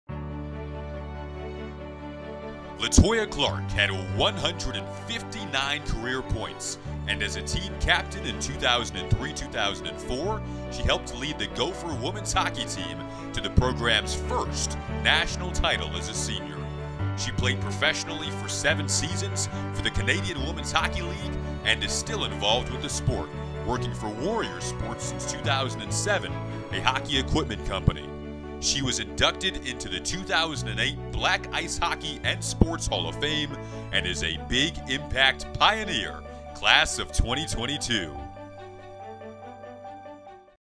B1G Impact Pioneer: Minnesota's LaToya Clarke
LaToya Clarke helped bring Minnesota its first national championship.

She played professionally and continues to work for the hockey industry.

Clarke is the latest member of the B1Gimpact Pioneers: Class of 2022.